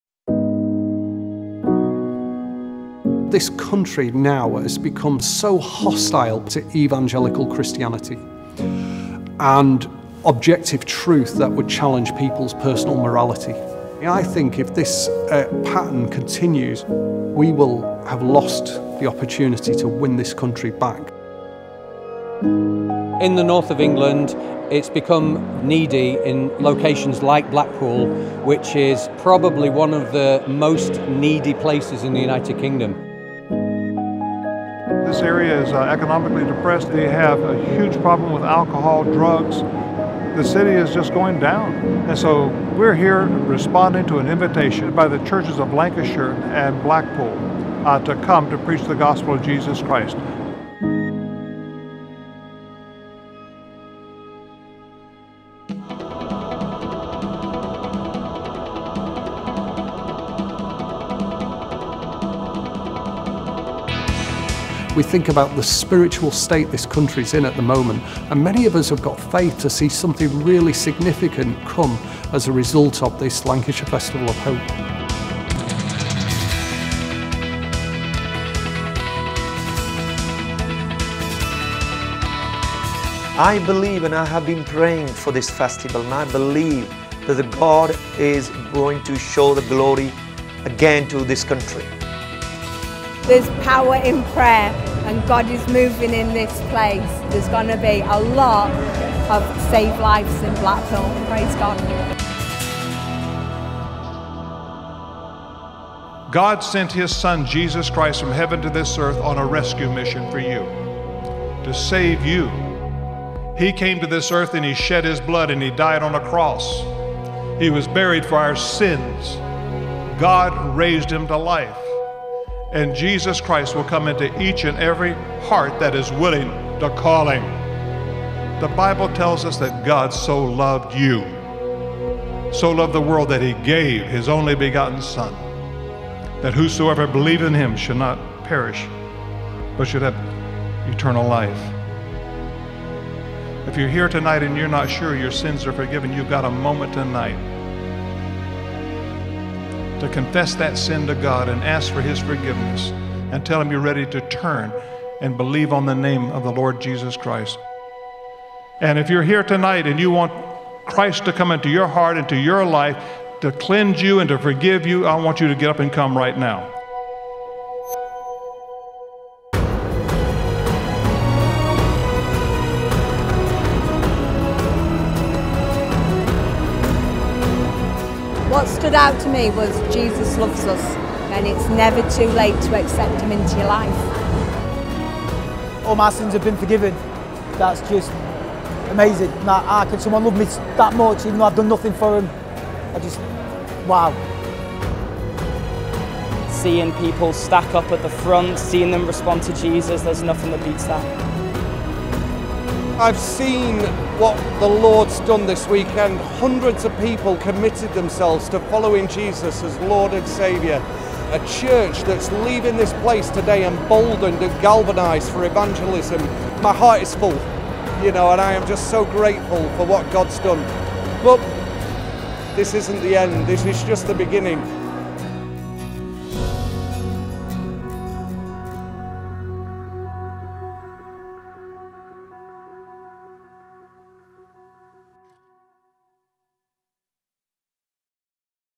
Thousands Hear the Gospel in Blackpool, England, Despite Opposition
Despite months of opposition, many hearts were impacted as Franklin Graham shared the light of the Gospel in Blackpool, England at the Lancashire Festival of Hope.